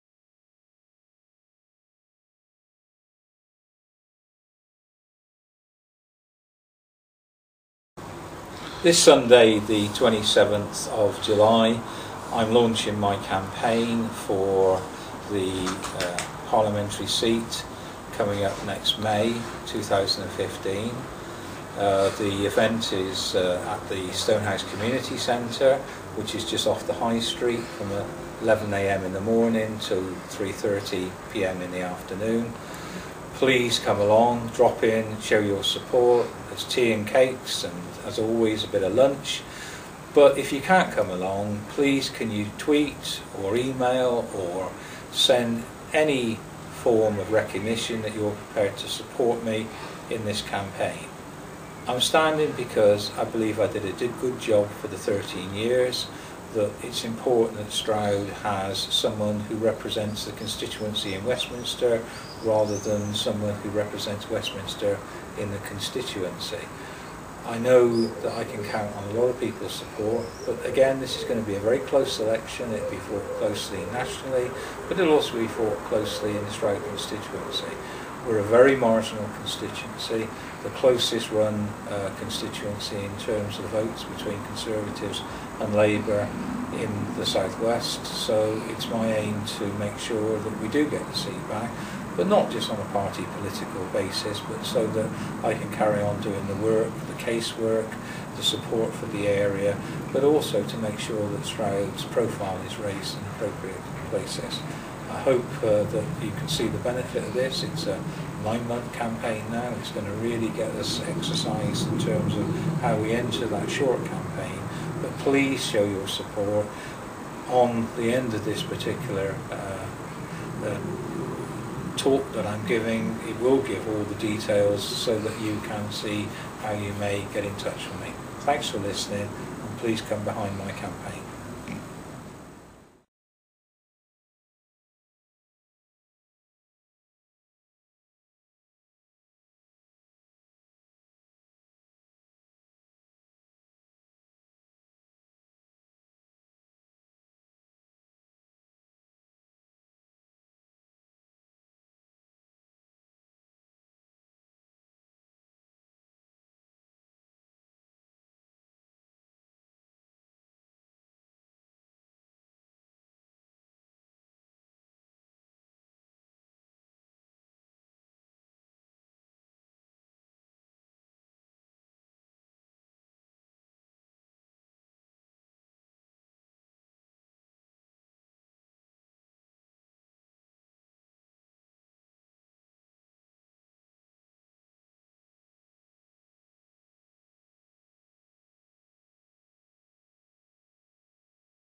David Drew Launch 27072014
David Drew launches his 2015 General Election campaign at Stonehouse Community Centre, Laburnum Walk, Stonehouse, Glos. on Sunday 27th. July 2014 at 12:30 pm. The centre will be open from 11:00 until 15:30 so drop by, pledge your support, have a bite to eat and talk with David.

If you cannot get to Stonehouse on Sunday you can pledge support via all electronic media.

Please tell your friends your acquaintances and all those that you know who support David Drew in his quest to regain the Parliamentary seat of Stroud. Help us to elect a Labour Government in May 2015 and also help us to elect Labour District councillors to give us greater control on the council